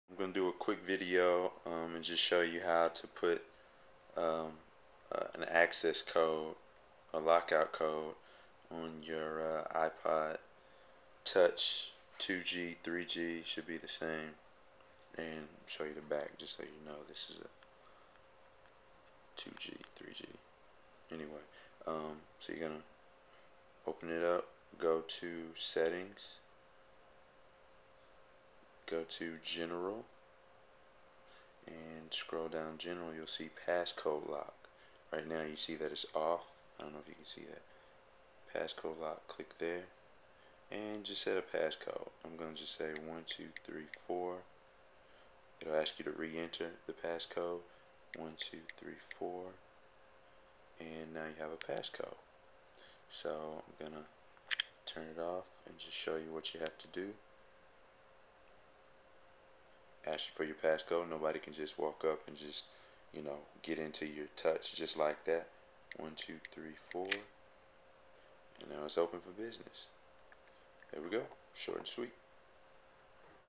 How to set a Security Code for your iPod Touch or Ipad
Good for ipod Touch, ipad, or Blackberry..